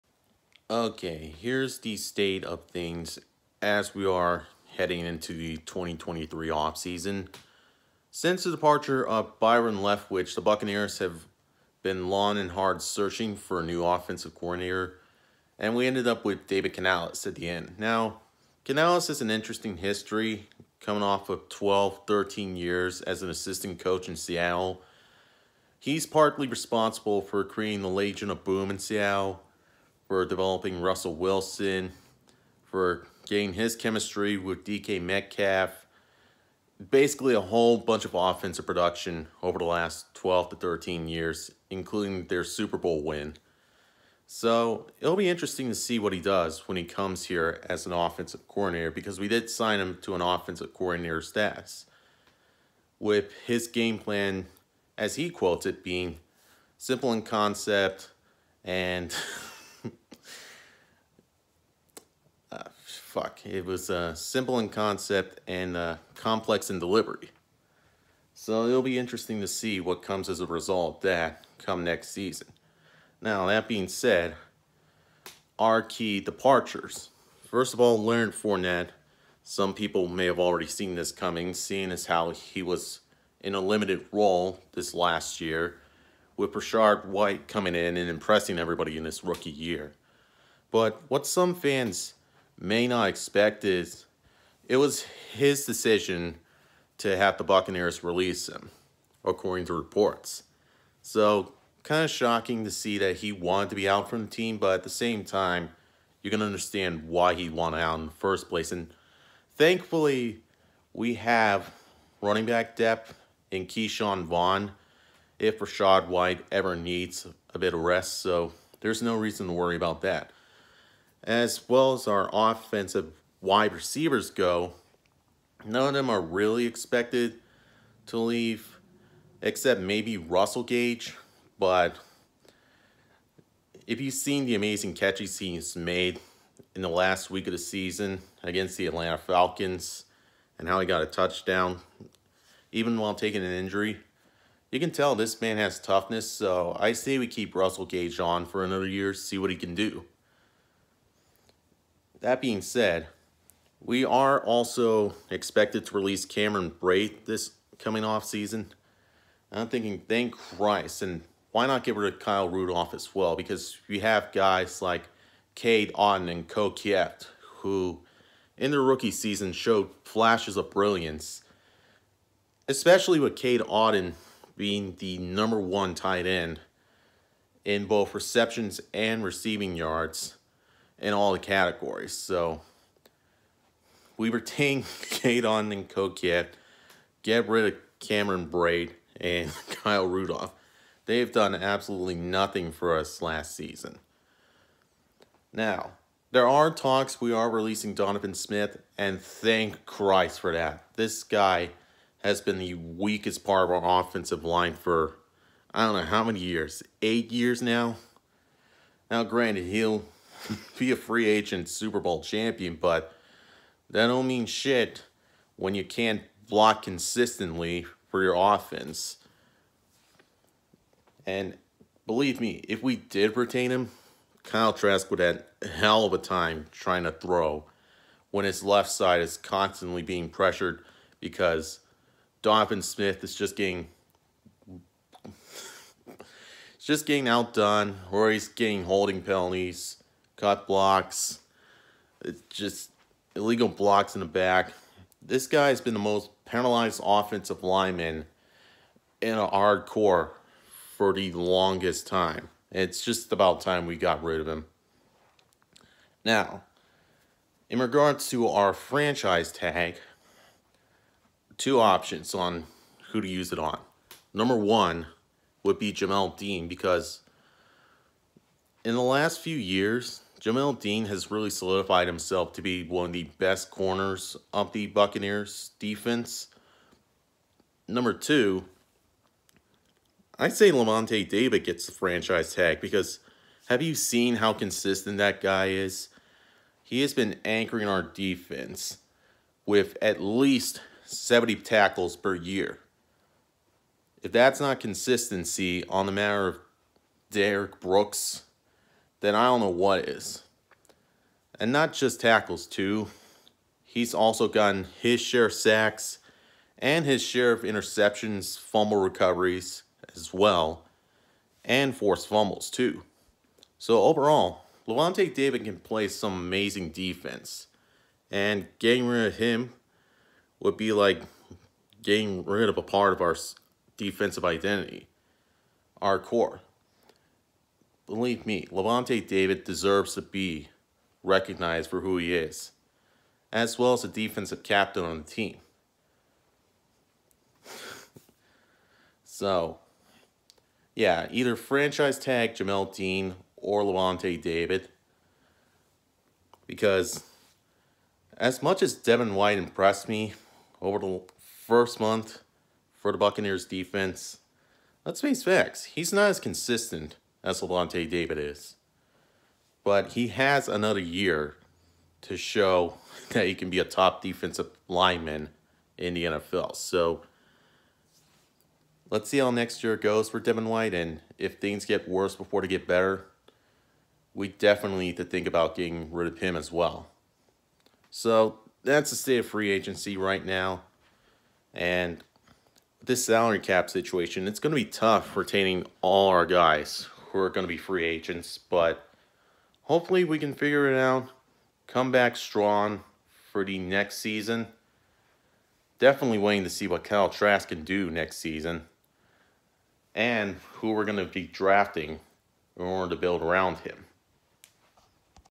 The State Of The 2023 Buccaneers heading into Offseason
So here’s a video going over how the Buccaneers are looking heading into this offseason, with the gains and losses we’re dealing with, as well as a small glimpse of who I think we should Franchise Tag heading into the offseason come free agency.